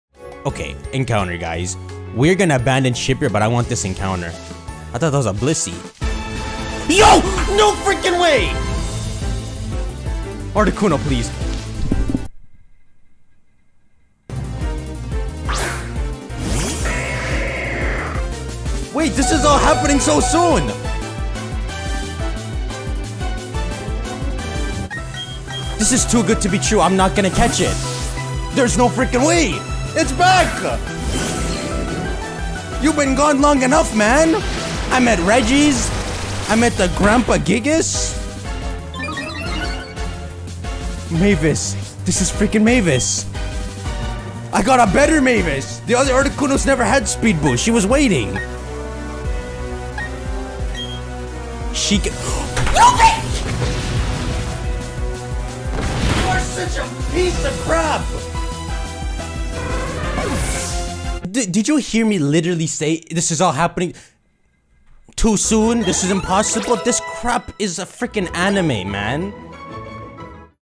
Articuno Returns
After catching Faris two episodes ago, Articuno gets revenge in a way that hits very close to home for TTar.